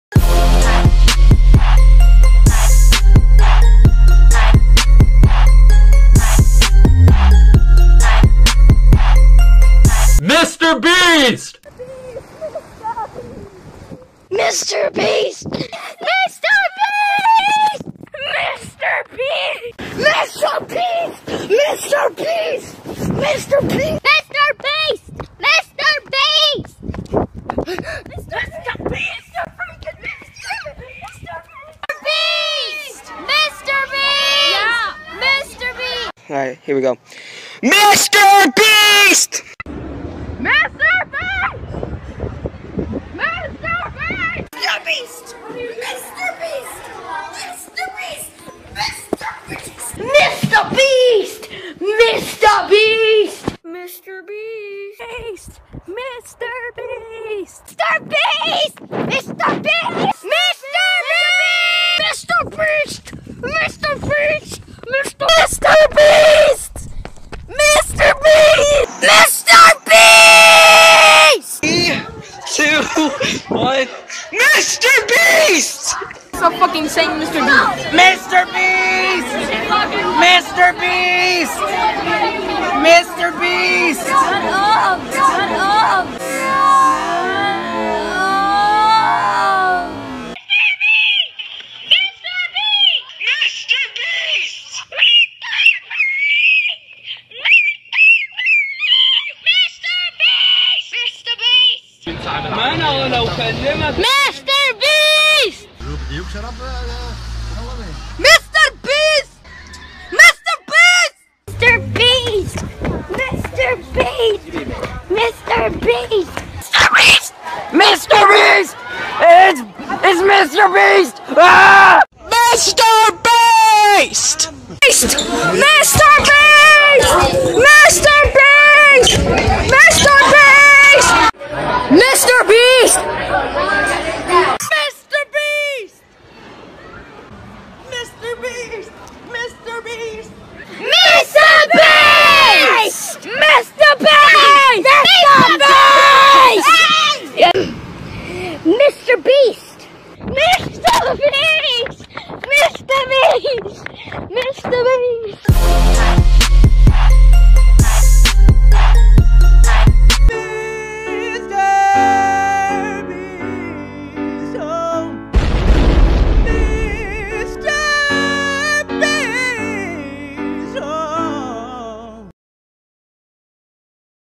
People Yelling MrBeast Compilation
MRBEAST!!!

Serious: Here you'll see People Yelling MrBeast for the sake of the old days. Unfortunately, there was NO compilation of these, so i had to D.I.M (Do It Myself)

Made by CS124